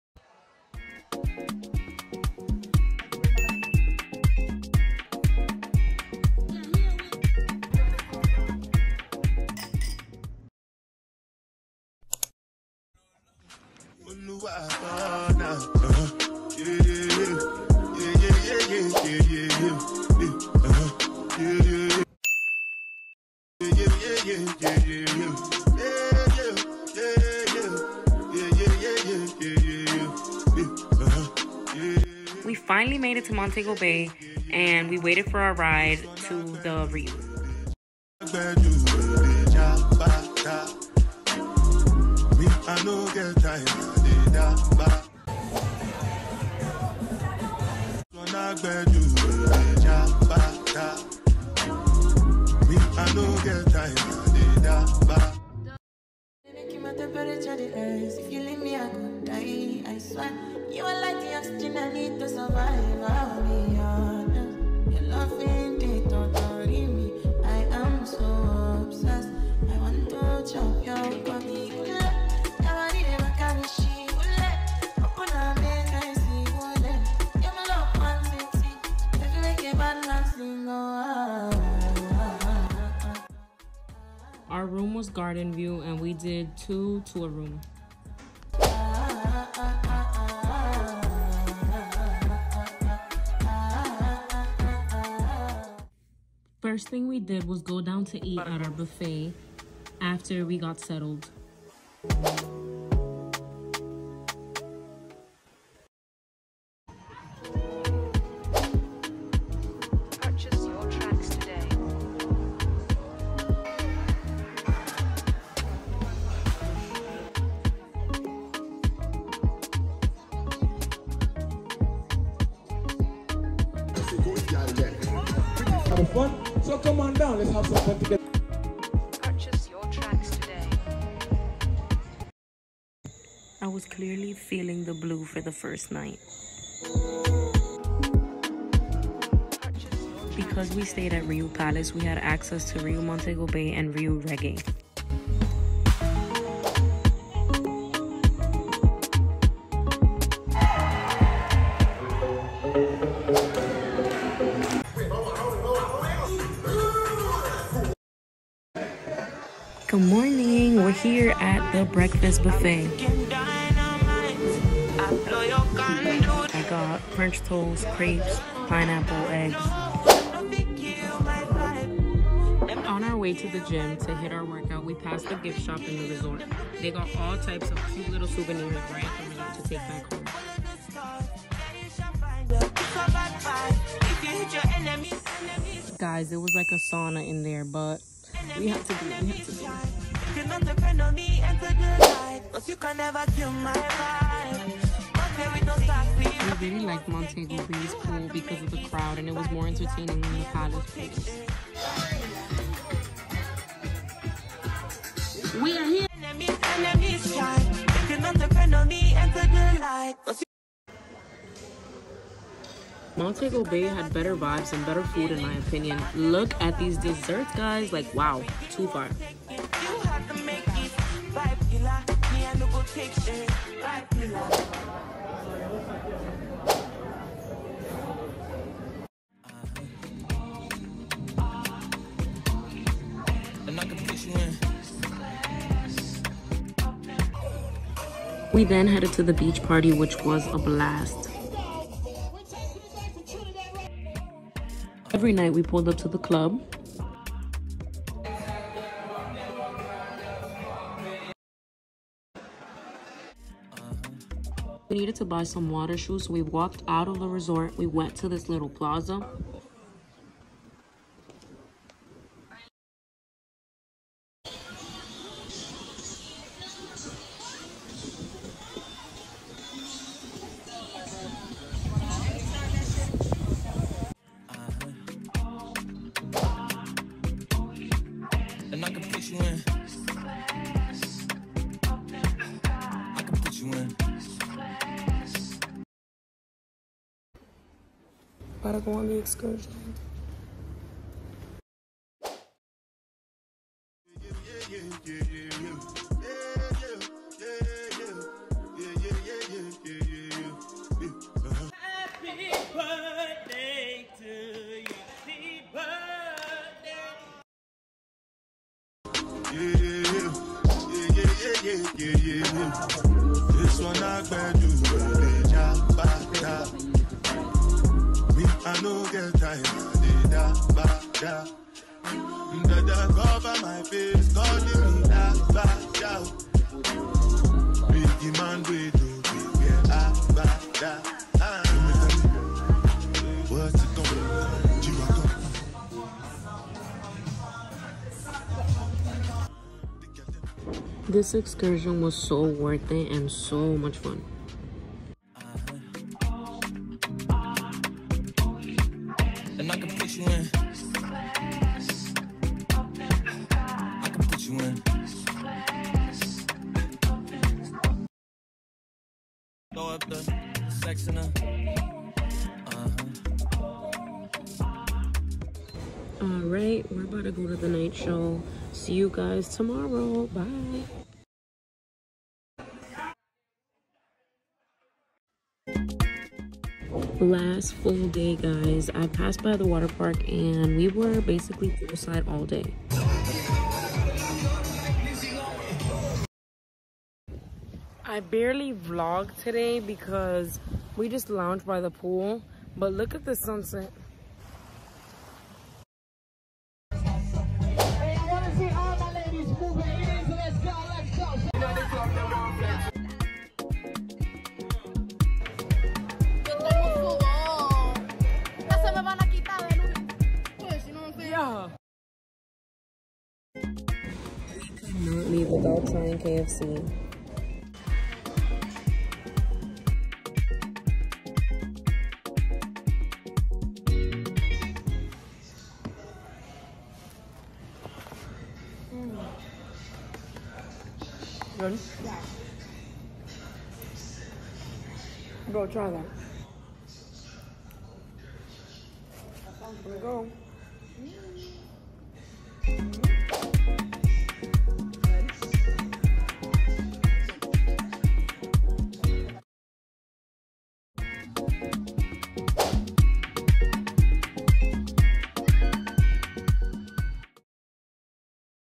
Jamaica Vlog: Girls Trip | Riu Palace Montego Bay, Bamboo rafting & more!
Hi y'all! Finally finished the video, this was a girls trip and birthday trip. We did our excursion with Chukka Excursions. :)